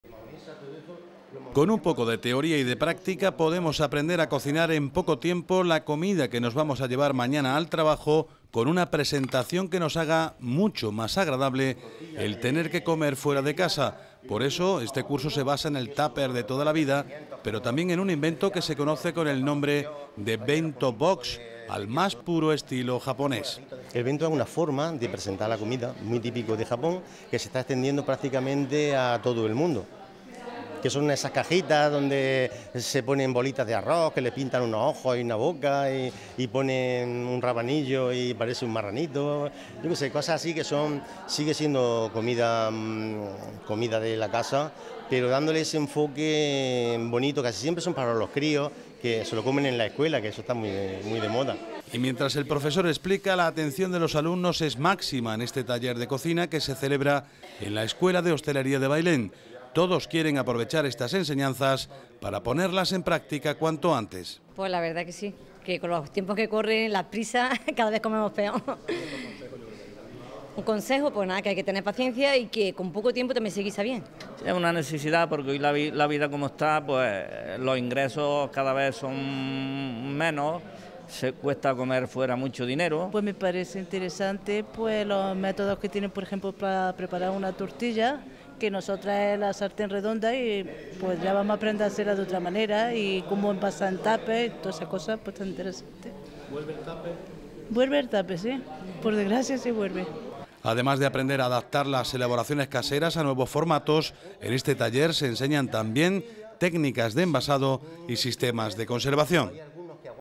UN CURSO DE COCINA EN BAILÉN PREPARA COMIDA PARA LLEVAR
Curso de cocina en Bailén para mostrar nuevas formas de preparar comida para llevar al trabajo o para el cole de los niños. Se trata de ahorrar y de aprovechar los ingredientes tradicionales, con nuevas formas de presentación.